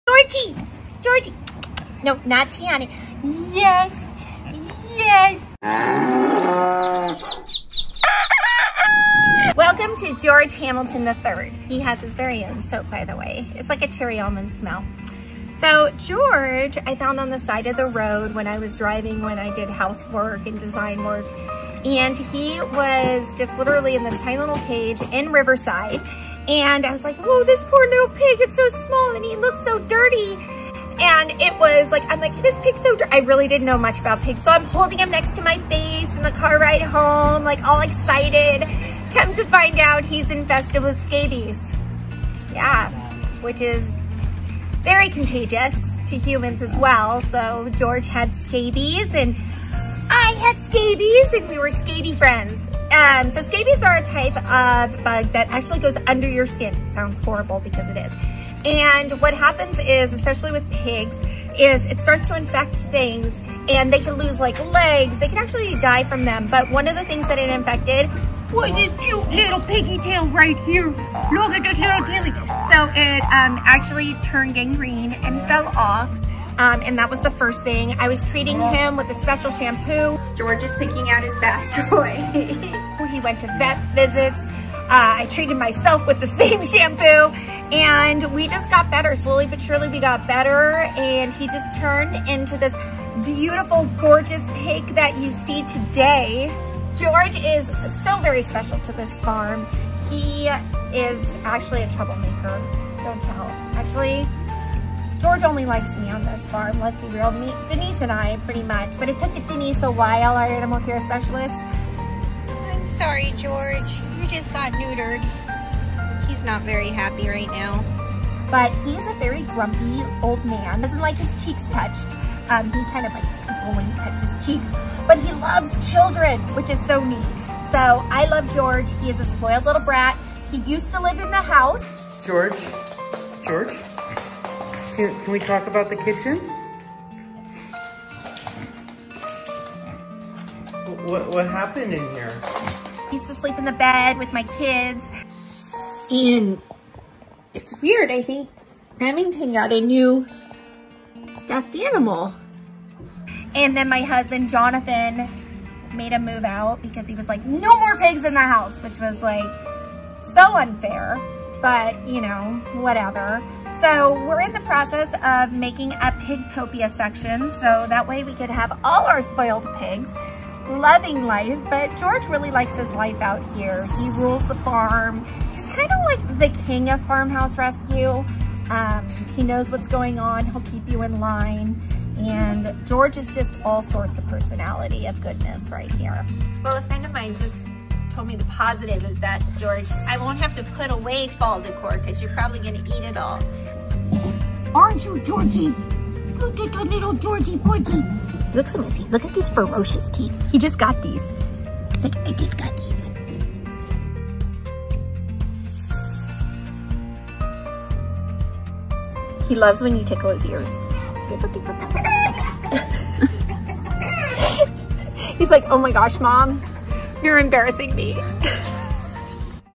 FINDING A PIG ON THE SIDE OF THE ROAD!? | George Hamiltons rescue story
George is the king of the farm and is loved by so many children. His story touches the hearts of all of our volunteers and we hoped it touched yours.